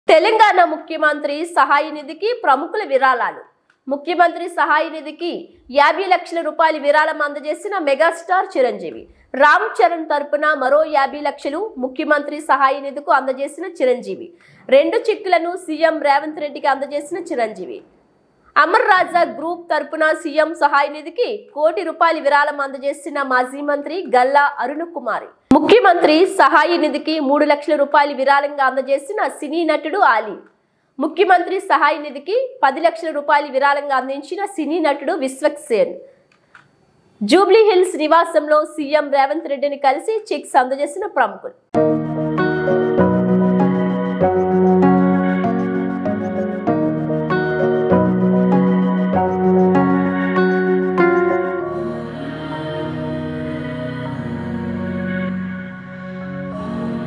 వరద బాధితుల కోసం విరాళాలు ఇచ్చిన ప్రముఖులు
తెలంగాణ సీఎం రిలీఫ్ ఫండ్ కి విరాళాల వెల్లువ